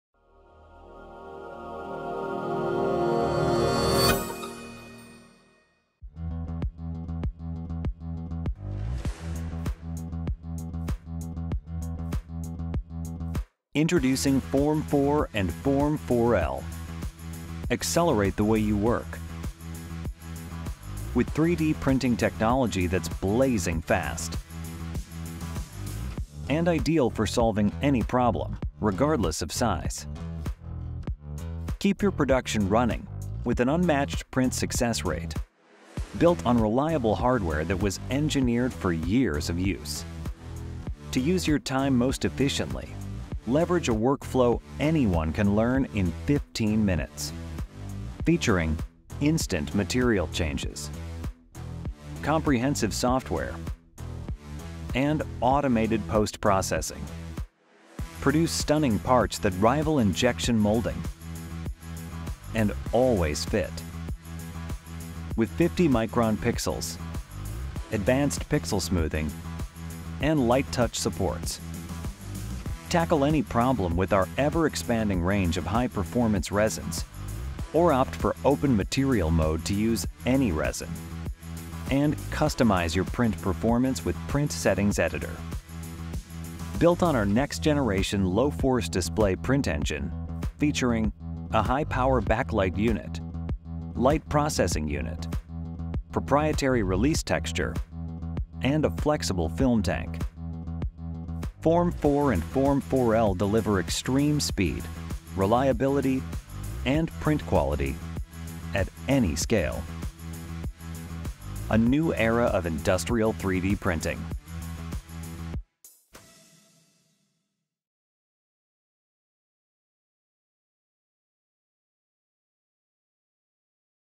Formlabs大尺寸列印機新世代 Form 4L，極大空間極速列印｜formlabs｜台灣天馬｜CC字幕
3D列印的國際領導品牌 Formlabs 今天推出新一代大尺寸 3D 列印機 Form 4L / 4BL ，為大型列印物件提供無與倫比的速度、可靠性和列印質量，使用戶能夠輕鬆列印大型原型和生產零件。Form 4L / 4BL的成型體積是Form 4的4.6倍，延續Form 4使用Low Force Display™ 列印引擎，有效結合超高功率LED燈、透鏡陣列、光學濾波器和模組化紋理液晶顯示器，大多數複雜的大型物件可以在6小時內列印，也與多達23種的Formlabs材料相容，另外，可透過選購Open Material License使用第三方材料，是多功能列印的全新終極解決方案。

更多消息請洽台灣授權代理商-台灣天馬科技股份有限公司。

【台灣天馬科技 聯絡資訊】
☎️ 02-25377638